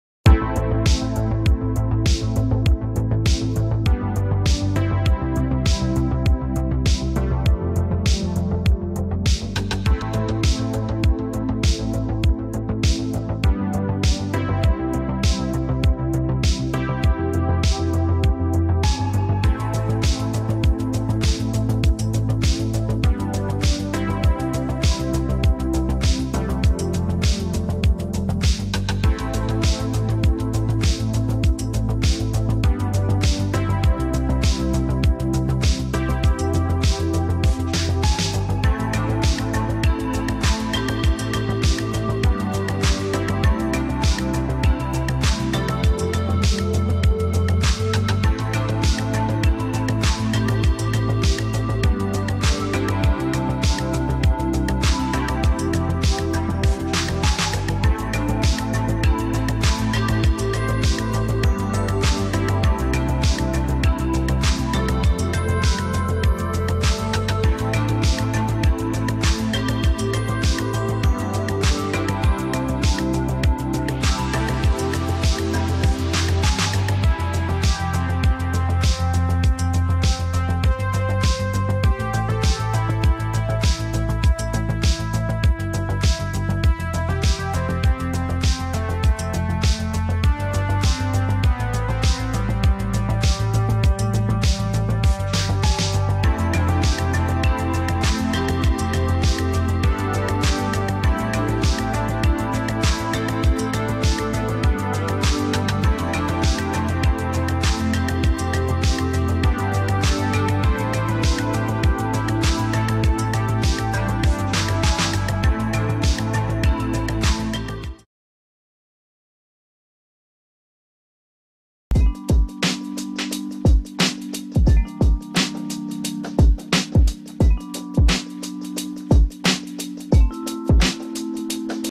Los virus que pusieron en riesgo la salud de todos en el 2020 | Telemundo Lifestyle
El COVID-19 hizo que el mundo cambiara por completo en este 2020. Sin embargo, durante estos meses la humanidad también se enfrentó a otros virus que surgieron principalmente en Asia. Mira cuáles son.